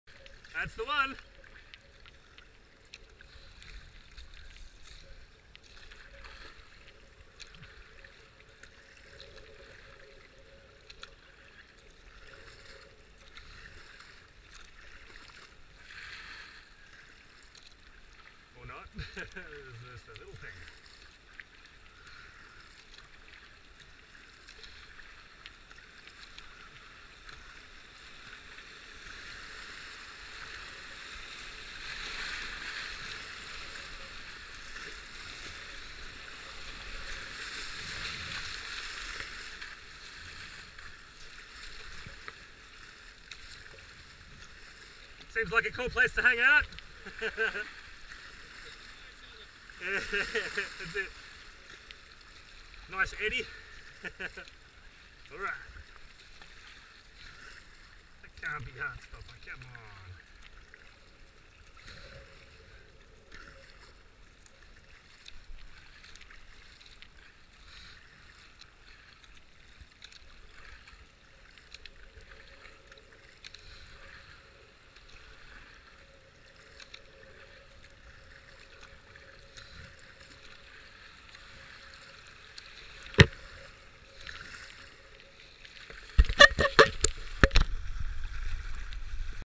Avon Descent 2022 - 33 Day2 Heartstopper
The predicted water level today was 1.3m at Walyunga. Levels were dropping even with the rain yesterday.